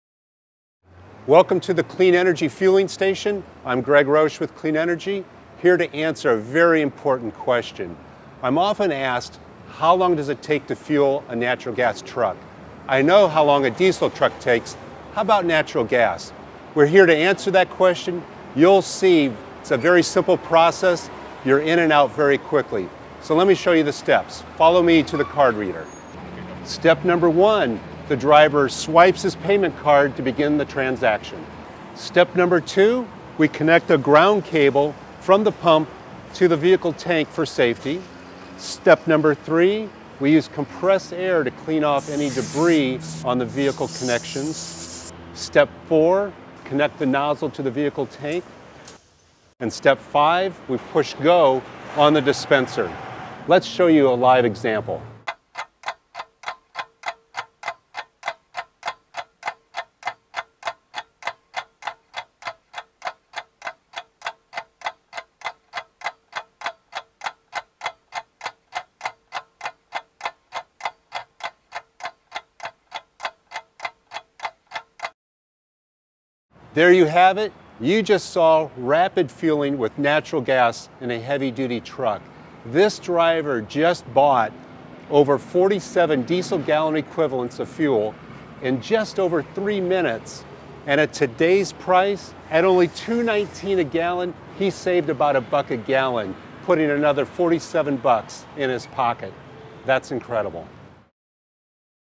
Fast Fueling an LNG Natural Gas Truck
CNG and LNG are the next big fuels for trucks in America. Natural gas is cheaper, cleaner, abundant and American. Natural gas is bettern than diesel, but there are differences. One question commonly asked is how long does it take to fuel? Watch this video to find out.